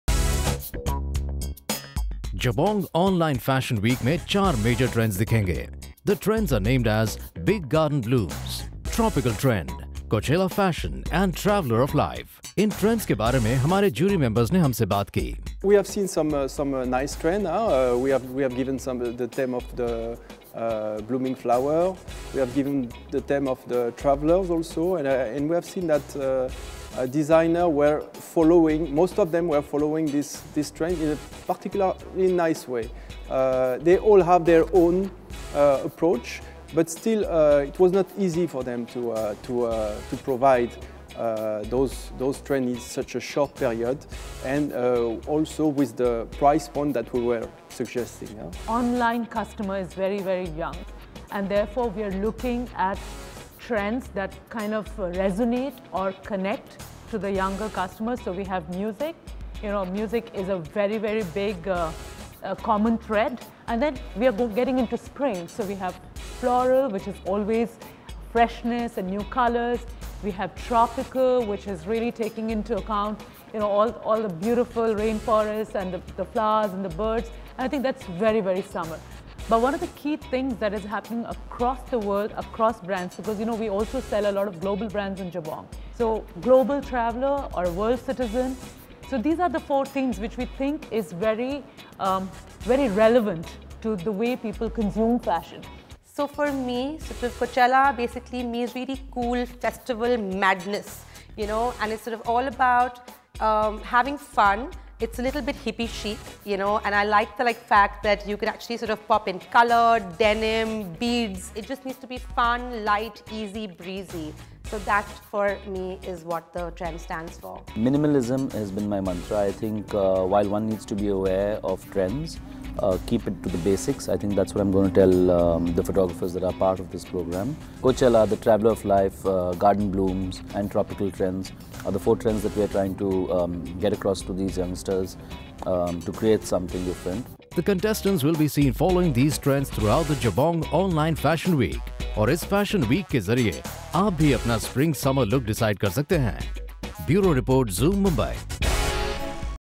Jabong Online Fashion Week's Trends!
Watch the big 4 trends at the Jabong Fashion Week

Serge Gianchandani : Head Production
Manjula Tiwari : Senior VP Women's Business
Nandini Bhalla : Cosmopolitian Editor
Coiston Julian : Fashion Filmaker